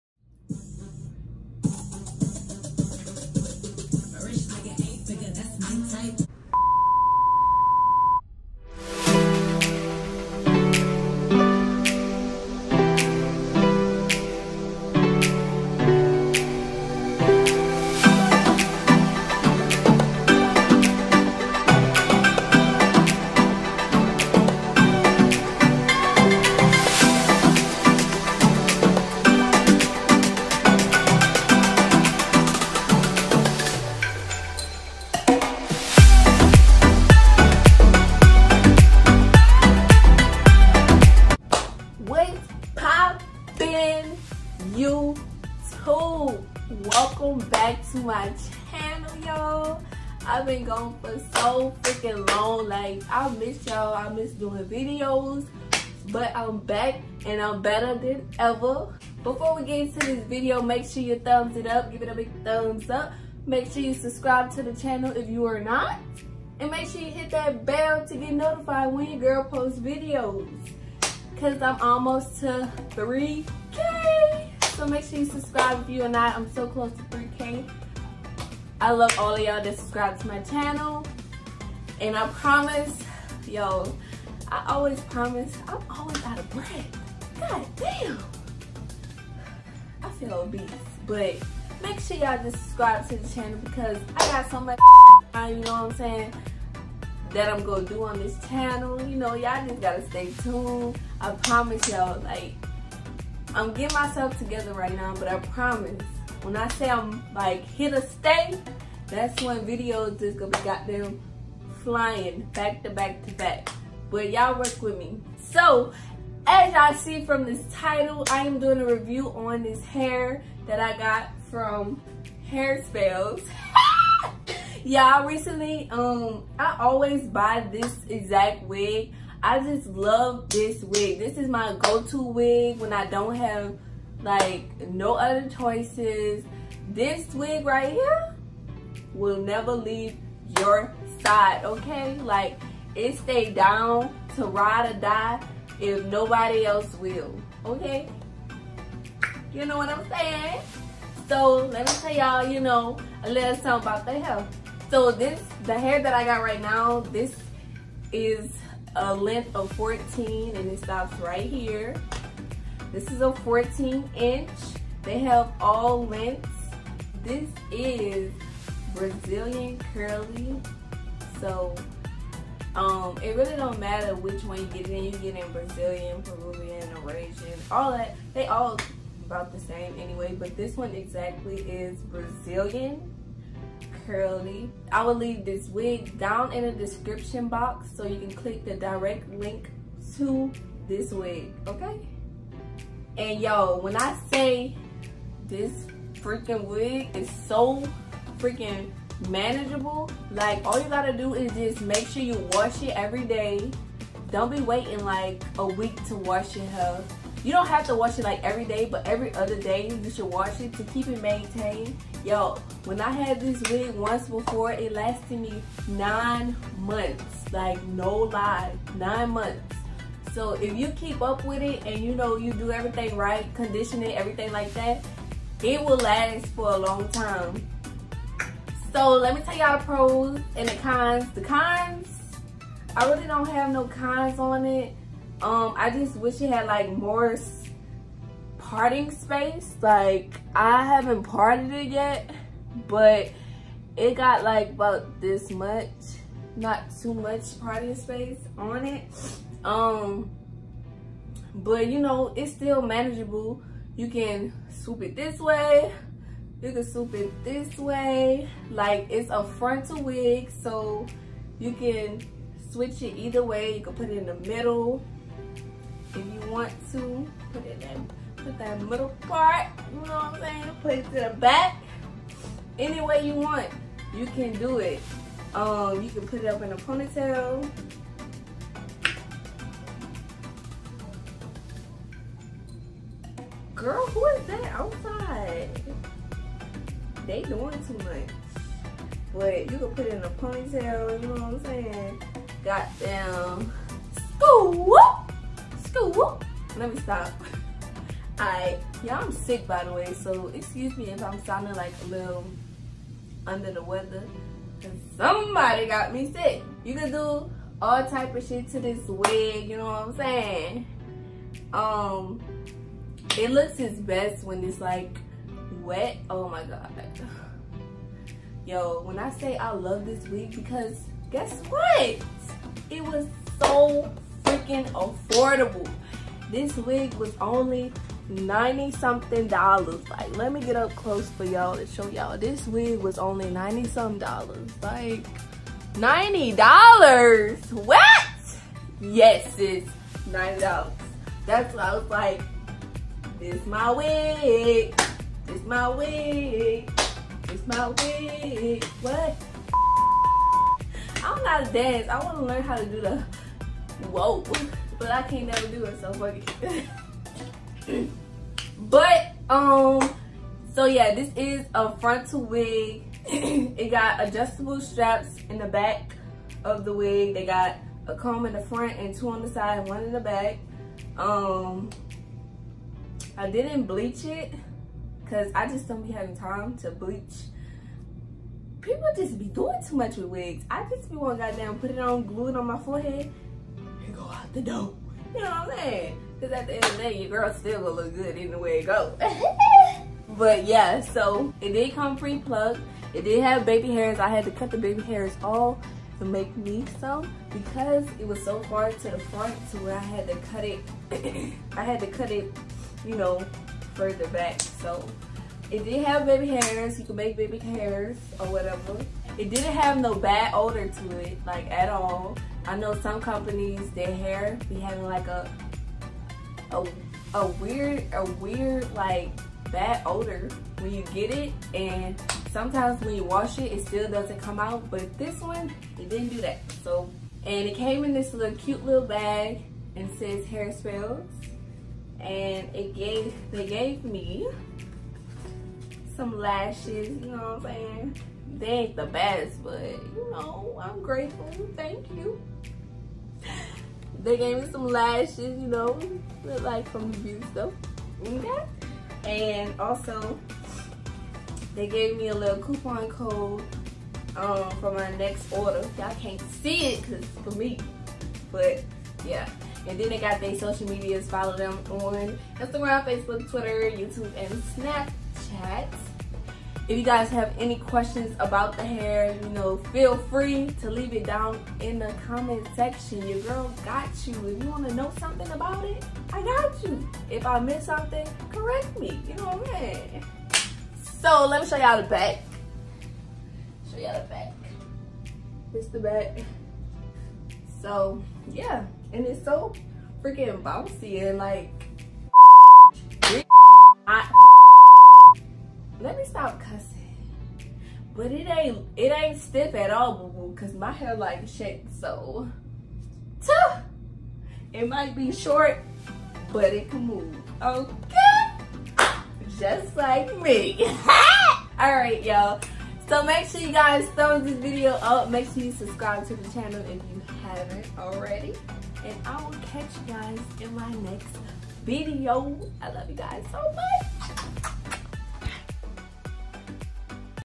Hairspells Lace Front Curly Wig Review | Not Sponsored
💁🏽‍♀️Hi y'all. I am back with another video. This curly wig just is so bomb. Please keep in mind that this wig was purchased on my own coins, which means this review isn't a sponsored video. Thanks for y'all watching!